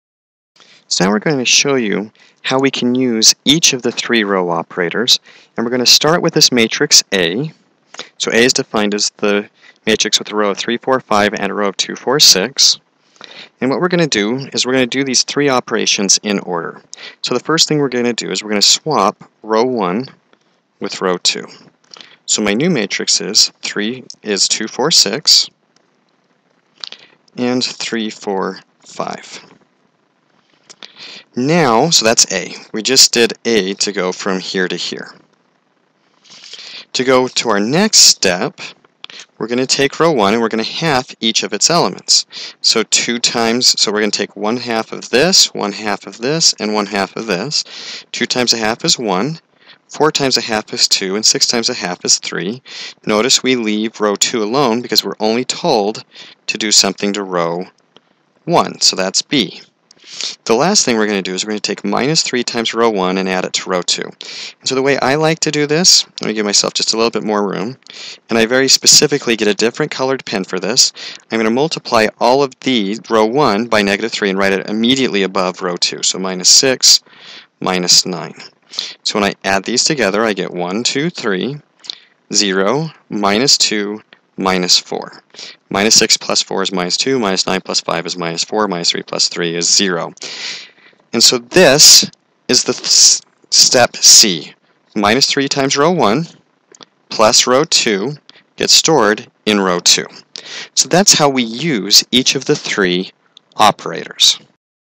Use Basic Row Operations
This video demonstrates how to use the basic row operations on a matrix.